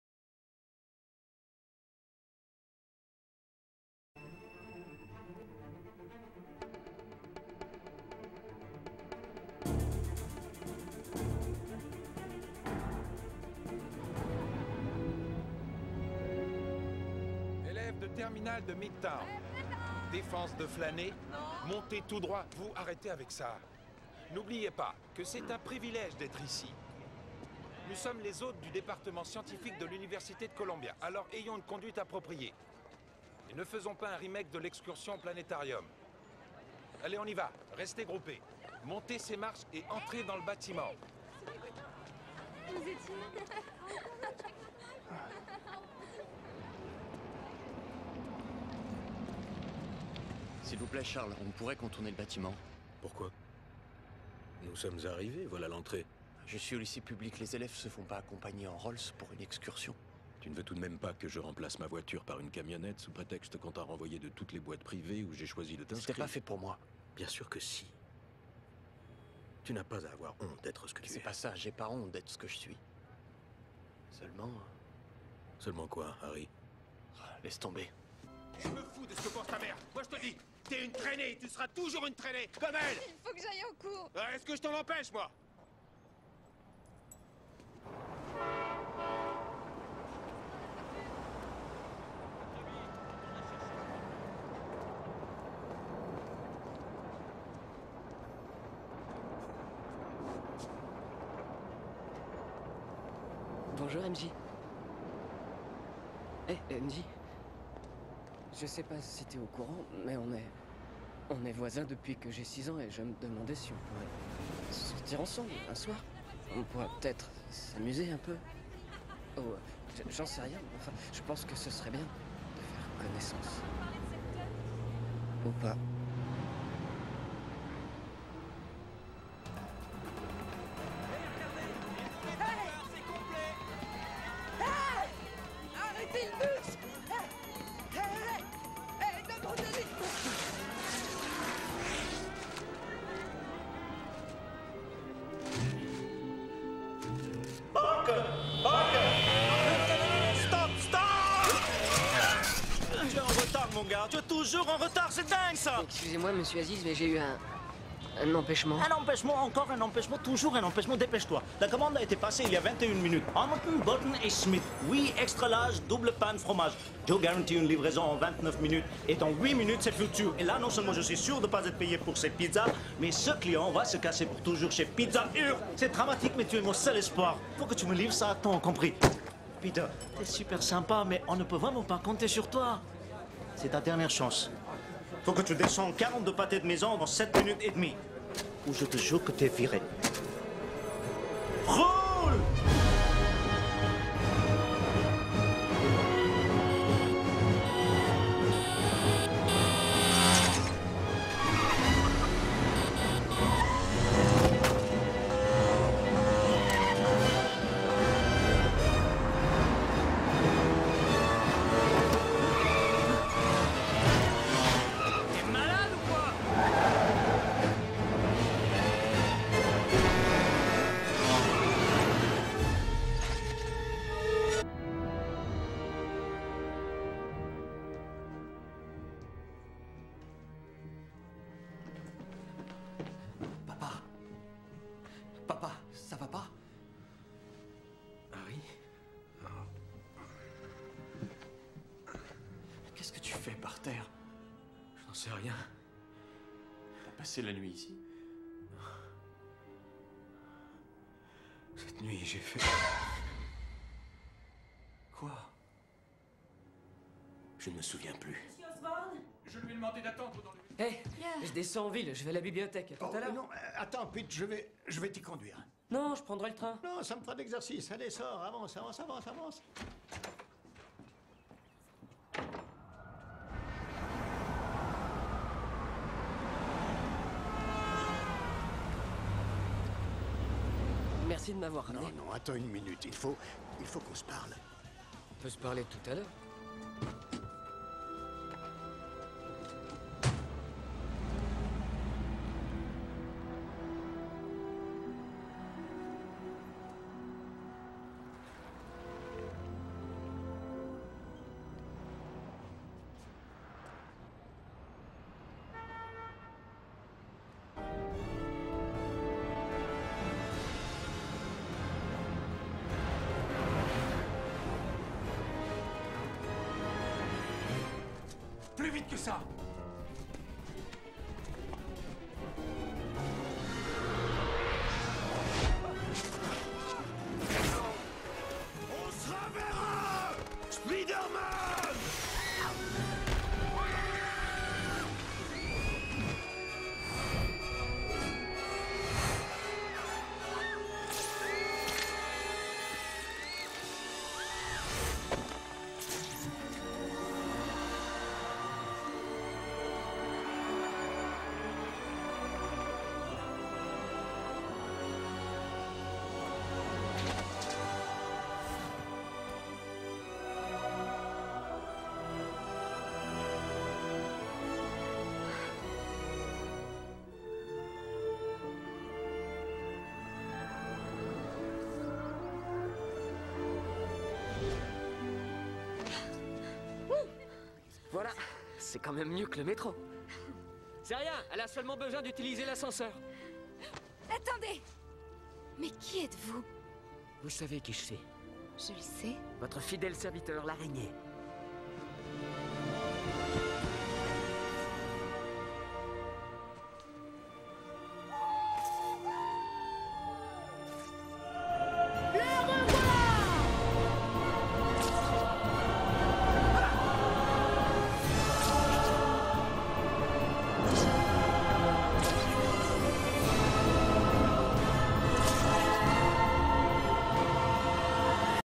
Habiter New York
Habiter une métropole de pays développé (É-U).
Programme de géographie de sixième.